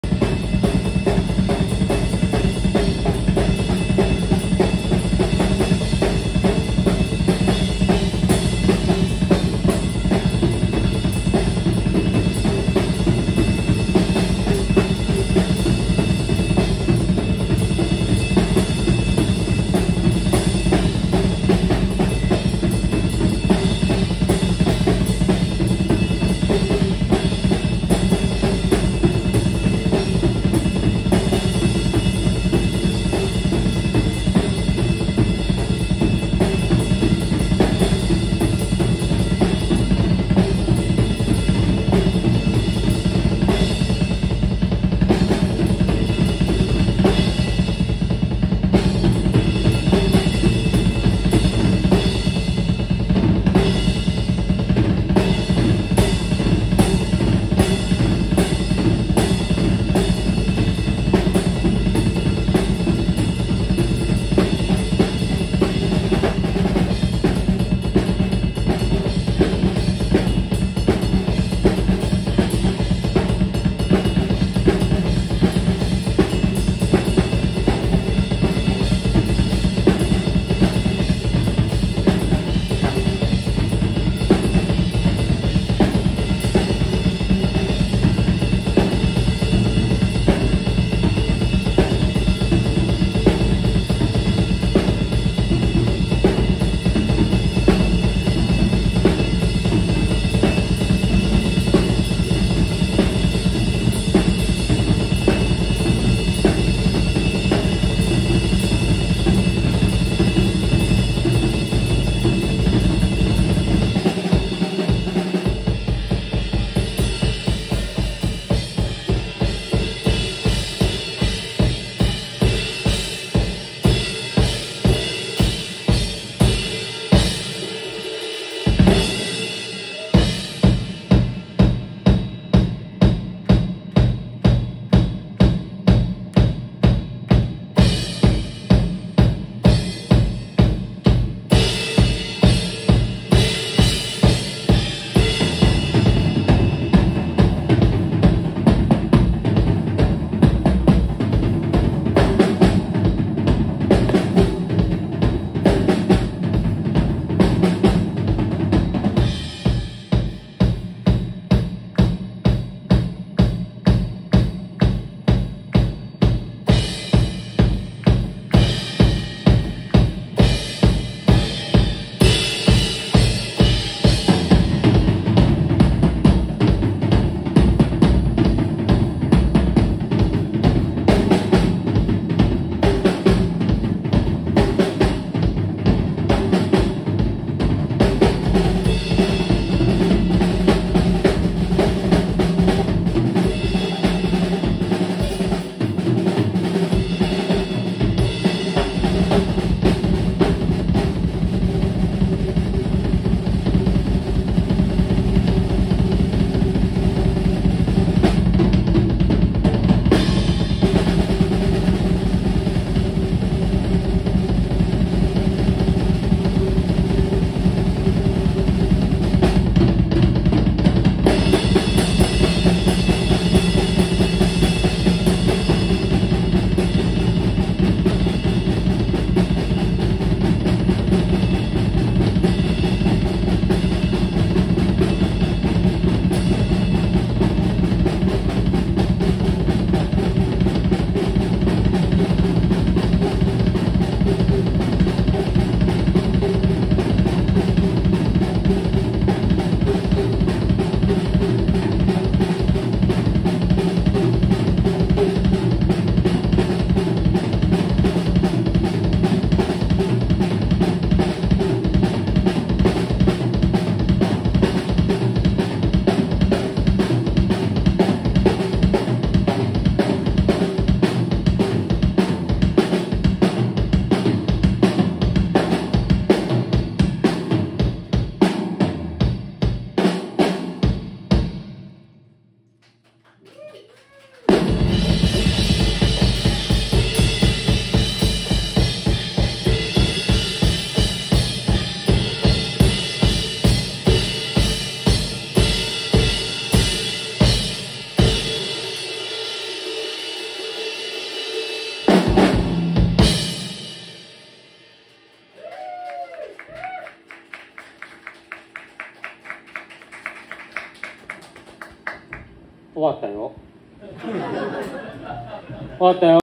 Masayuki Osada Drum Solo / 2024/9/15(sun)Tetsuya Mitani's AMADEUS@阿波座BIG JACK
Tetsuya Mitani's AMADEUSの2024年9月15日の2部構成ライブより1部のドラムソロ。